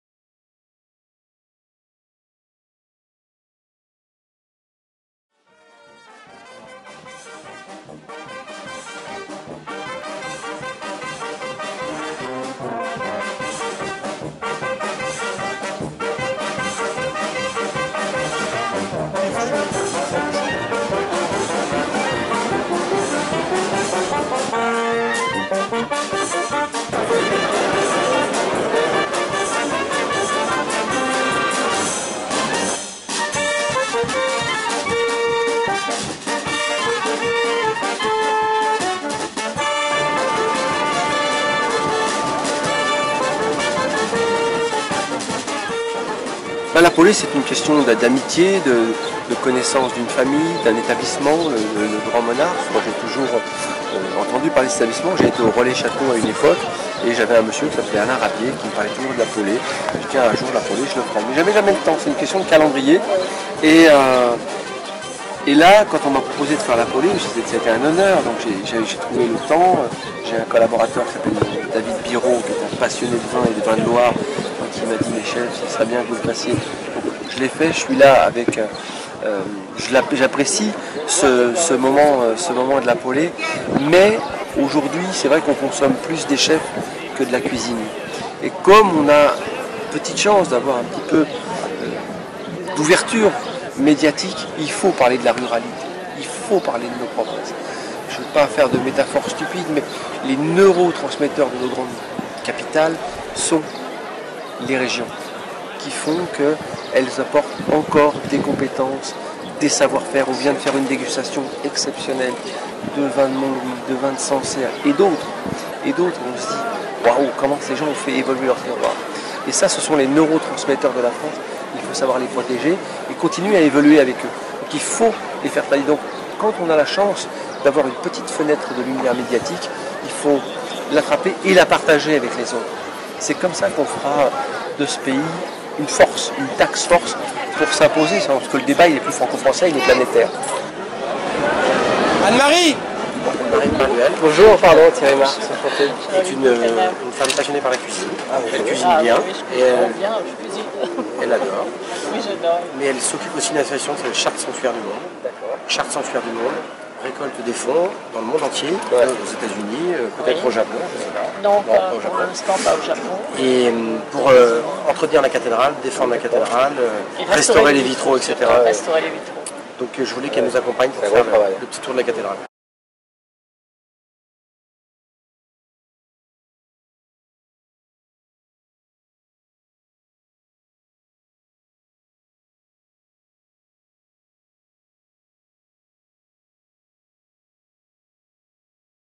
Paulée des Vins de Loire 2018 - A vos Marx #1
La Paulée des Vins de Loire 2018 s'est déroulée le dimanche 7 avril 2018 à Chartres. En journée, les vignerons du Val de Loire étaient réunis Place Billard pour le 15ème marché de la Paulée des Vins de Loire. Thierry Marx était l'invité d'honneur de cette édition.